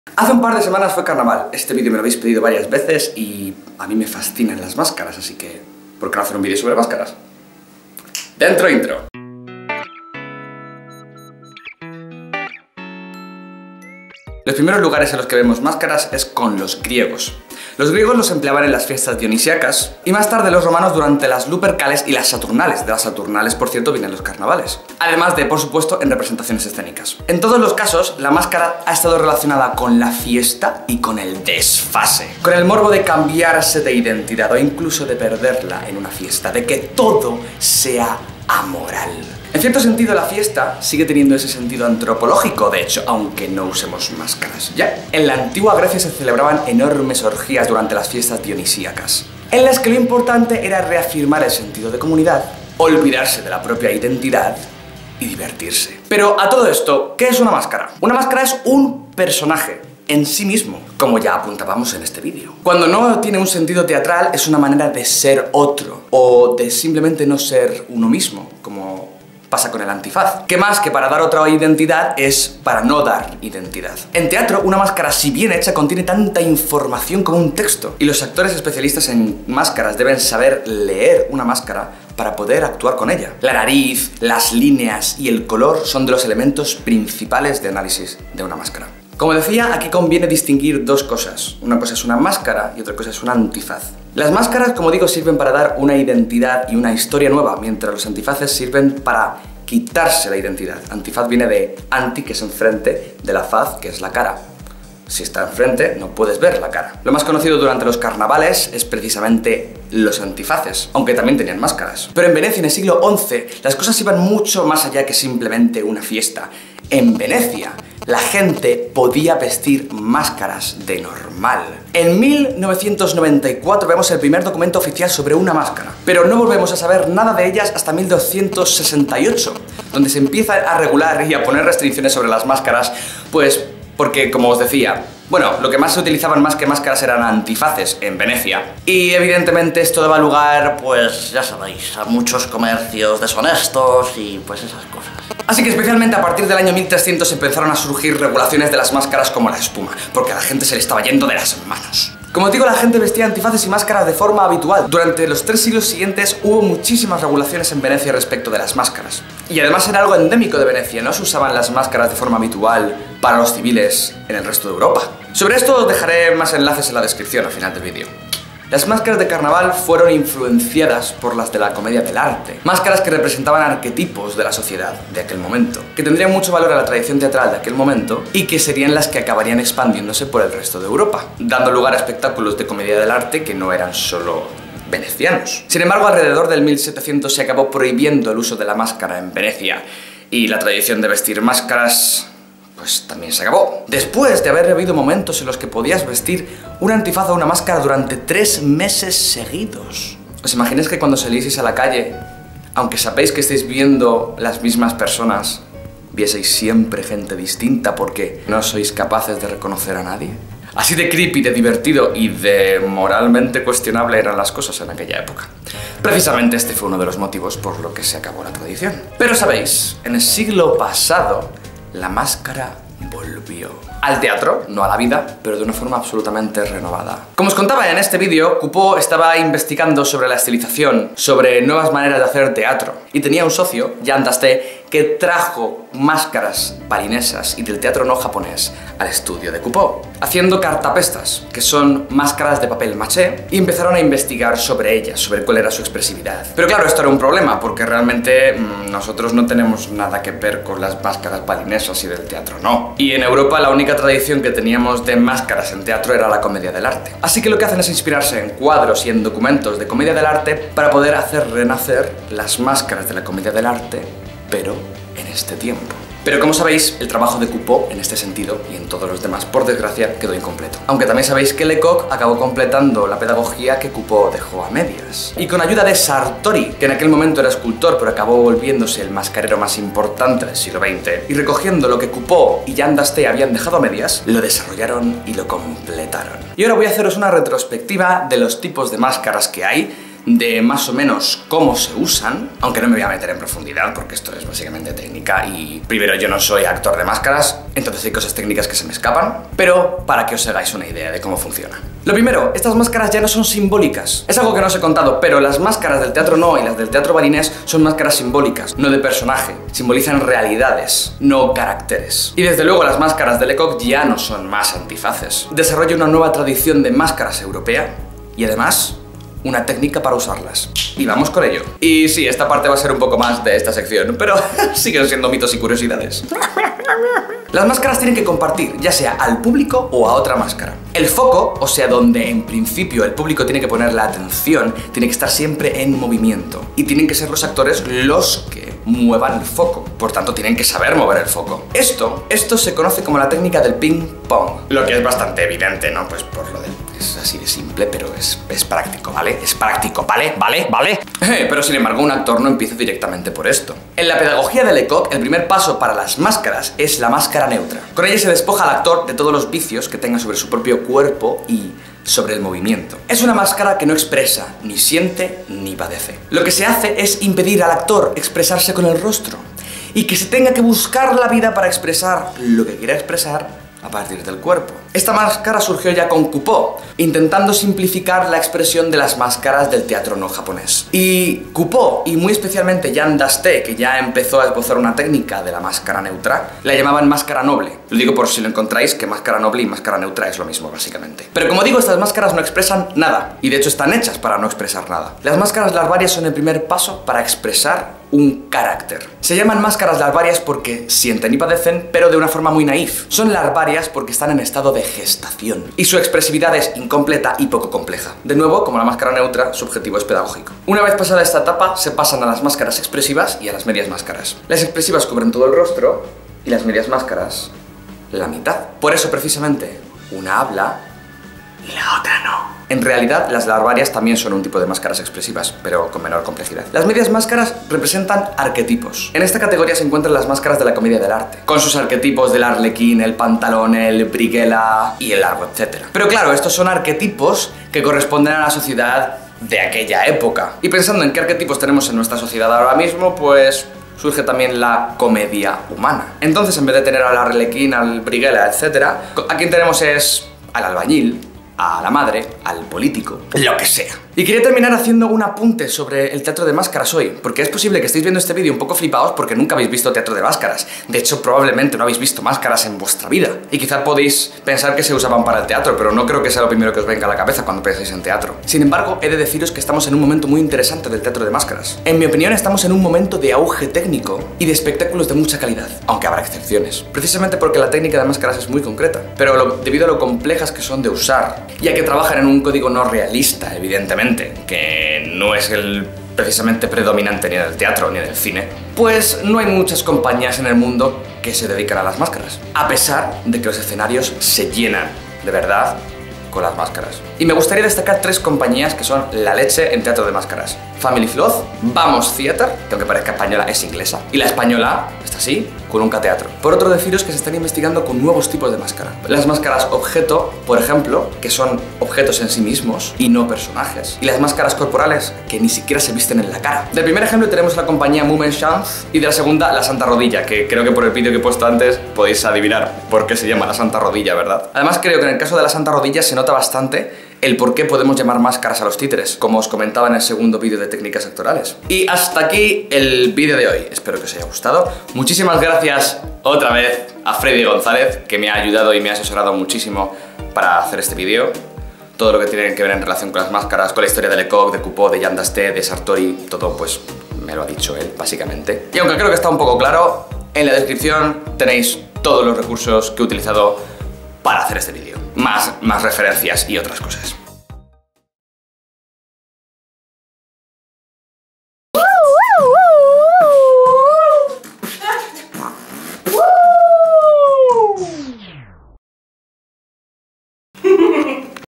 MÁSCARAS en TEATRO (Especial Carnaval) | Mitos y Curiosidades
Hoy hablamos sobre máscaras. El origen de ellas, cómo se crearon, cómo se usaban durante el Carnaval de Venecia, para qué se usa y qué es realmente una máscara. Nos metemos de lleno en cómo el teatro rescató a las máscaras del olvido.

¡Nos vemos en el siguiente ensayo!

- LIBRO: El cuerpo poético. Jaques Lecoq.
- LIBRO: El arte del actor del siglo XX. Borja Ruiz.

¡Vídeos cada semana!

¡Sígueme!:
TWITER e INSTAGRAM: @ensayarte